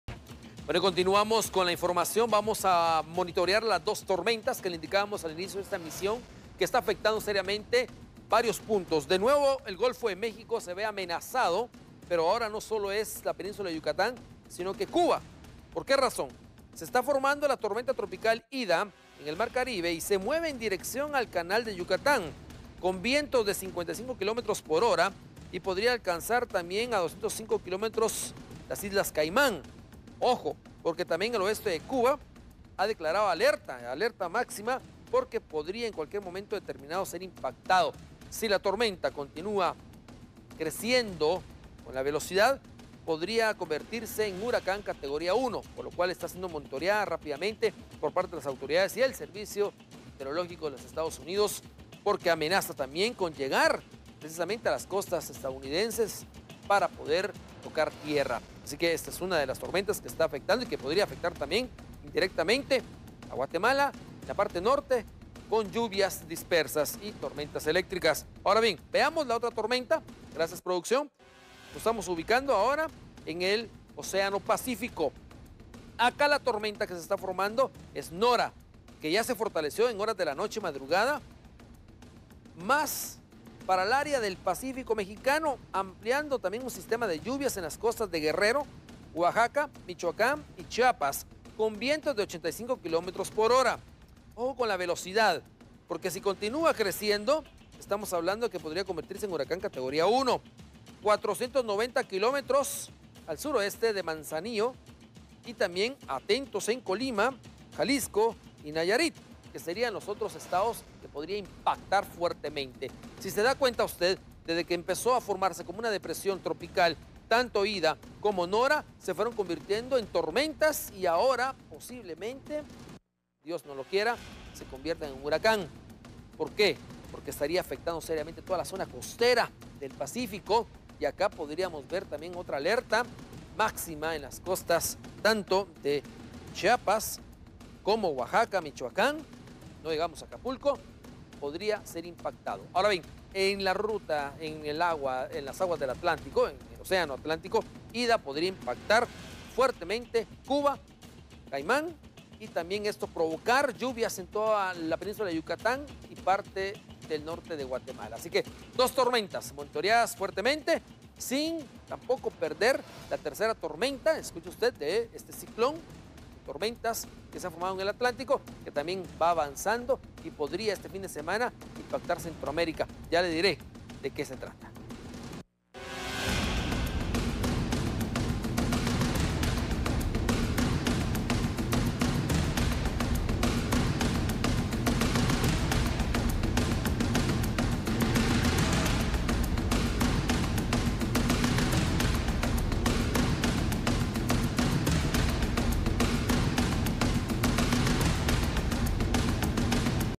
Dos tormentas afectan costas del Atlántico y Pacífico
Ambos fenómenos  son potencialmente dañinos

En las costas del norte, estaría la tormenta Ida. Mientras que en la zona del Pacífico oriental, estaría Nora que afectaría a las costas de México. Visita nuestro sitio